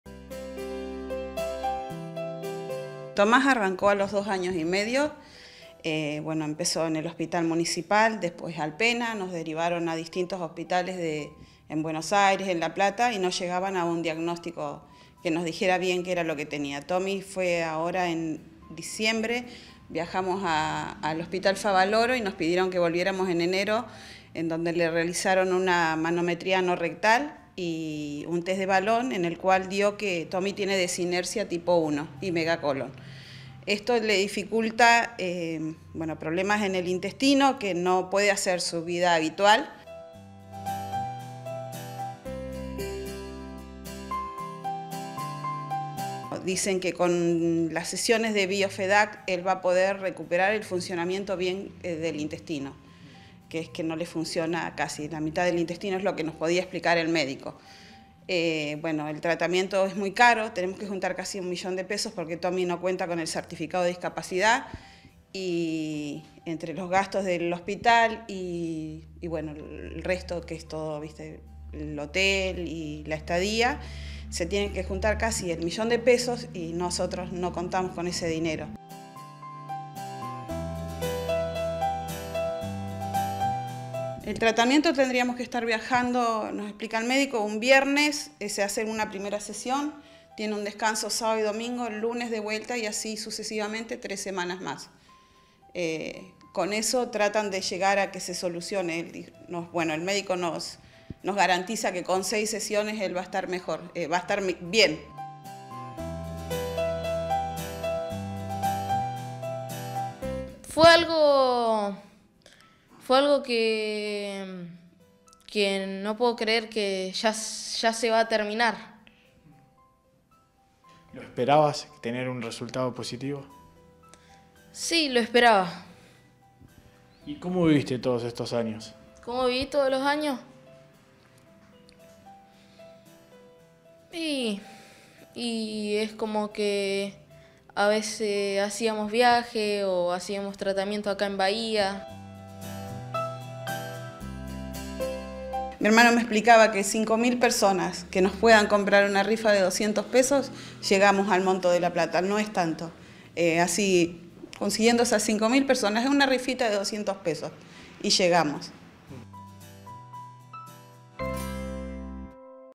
Tomás nos necesita: Debe recaudar $1 millón para un tratamiento que cambiará su vida
Tiene 11 años y desde los 2 sufre problemas en el intestino. Debe viajar a la Clínica Favaloro y necesita el dinero para costear el medicamento y la estadía de su familia. “Con 5 mil personas que nos compren una rifa de $200, llegamos”, sostuvo Alejandra, su mamá.